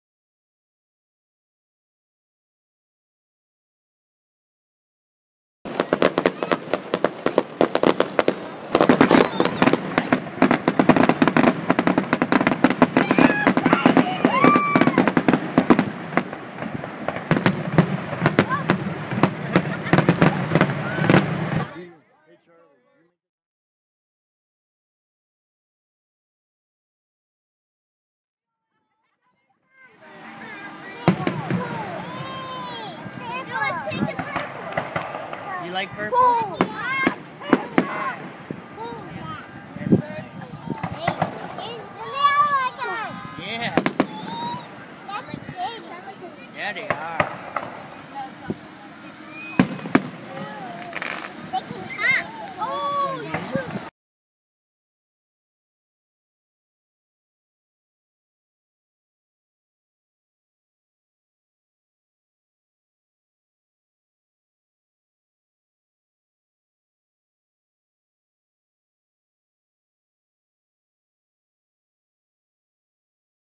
Fireworks over Middleburg Heights
We all jumped in the Mencini Van to see fireworks... yea!  Just to mad my video battery died... :(